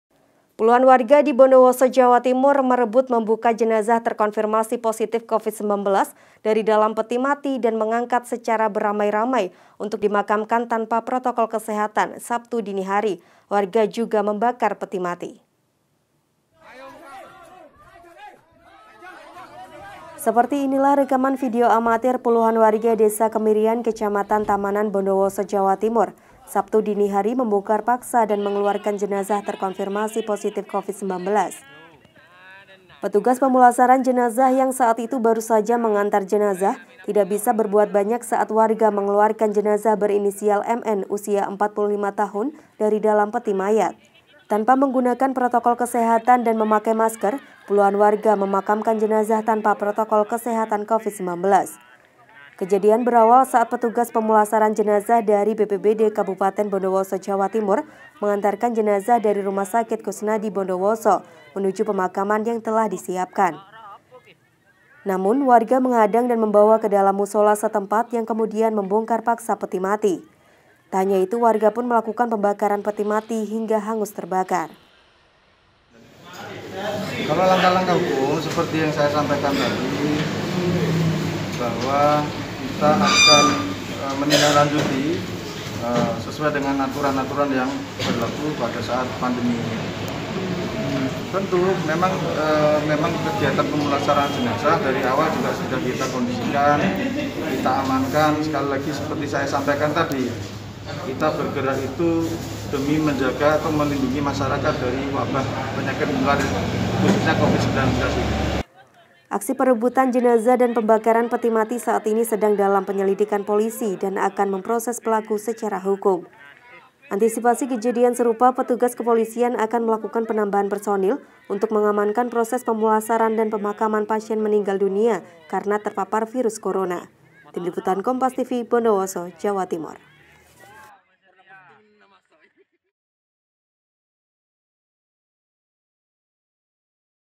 Warga Rebut Paksa Jenazah Covid-19 Dan Bakar Peti Mati
BONDOWOSO, KOMPAS.TV - Puluhan warga di Bondowoso, Jawa Timur, melakukan aksi pengambilan paksa jenazah pasien covid-19.
Mereka membuka peti, kemudian memakamkan jenazah dengan cara normal.
Kejadian bermula ketika ambulans petugas membawa jenazah pasien yang terkonfirmasi covid-19 dari RSUD Dokter Koesnadi, menuju pemakaman yang sudah disediakan.
Namun, warga kemudian menghadang ambulans dan mengambil paksa peti jenazah.
Warga kemudian, membuka peti, dan memakamkan jenazah tanpa memakai protokol covid-19.
Setelah mengeluarkan jenazah, warga kemudian melakukan pembakaran peti mati hingga hangus terbakar.
Aksi perebutan jenazah, dan pembakaran peti mati saat ini sedang dalam penyelidikan polisi.
Antisipasi kejadian serupa, petugas kepolisian akan melakukan penambahan personel untuk mengamankan proses pemulasaraan dan pemakaman pasien meninggal dunia karena terpapar covid-19.